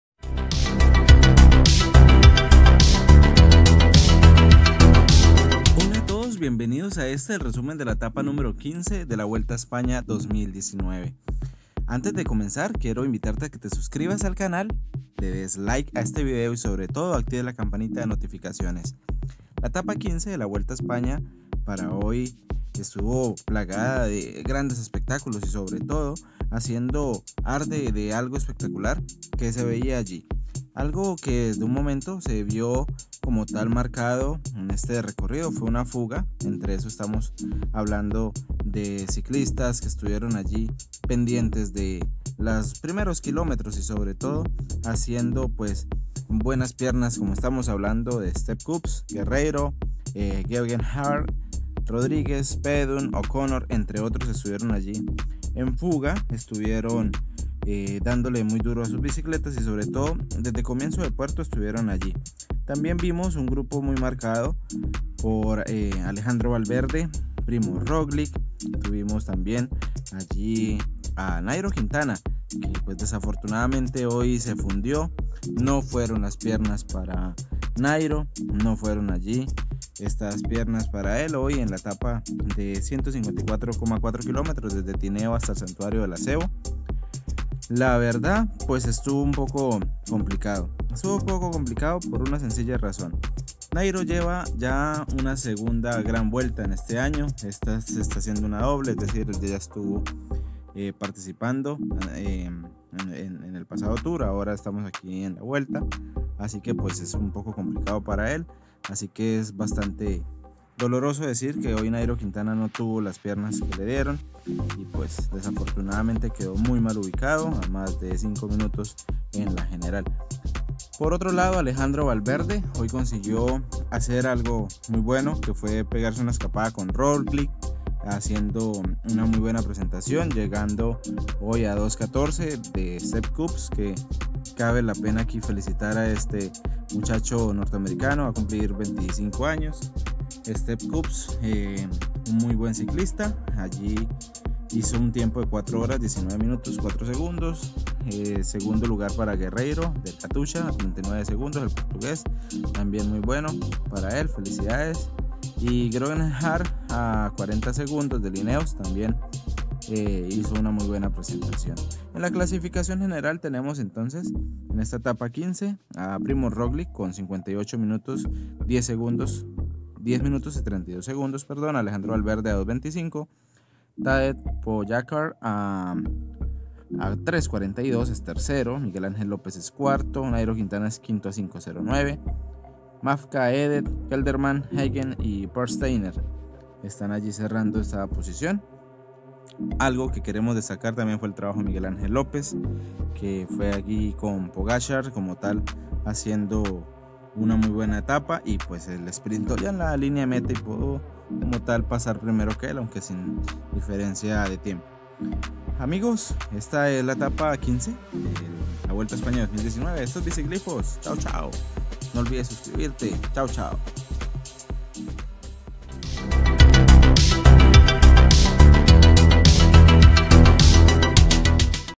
Resúmen 🇪🇸 ETAPA QUINCE🔥 Vuelta España 2019
Etapón de La Vuelta España 2019, hoy vimos en los últimos kilómetros como se firmó con sello dorado a menos que algo antideportivo ocurriera, como Primoz Roglic será el Futuro Campeón de La Vuelta España 2019.

Sepp Kuss el Norteamericano ganó la etapa 15 de La Vuelta, además vimos un Superman López atacar a Pogacar por el tercer lugar.

Alejandro Valverde atacó a Roglic, pero el esloveno parece imbatible y Nairo Quintana le pasó factura el cansancio acumulado, ya no tenía piernas.

⬆️Y mantente conectado al ciclismo⬆️